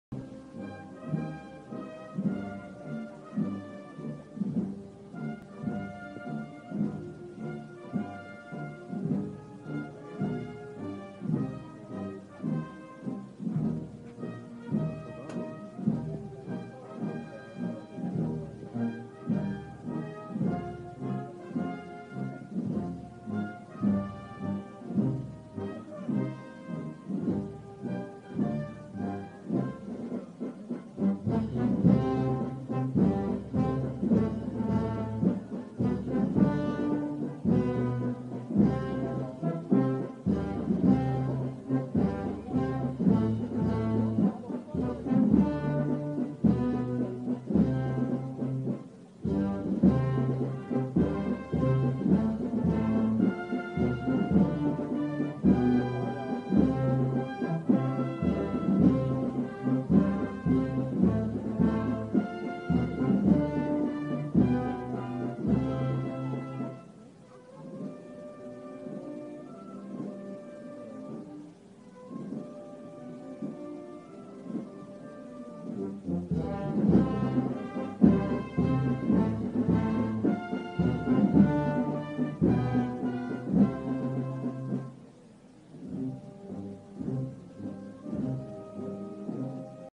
María Santísima de la Angustia - Los Estudiantes Sevilla 2017
María Santísima de la Angustia de la hermandad de los Estudiantes de Sevilla.
Acompañamiento musical: Asociación Musical Nuestra Señora del Águila (Alcalá de Guadaira).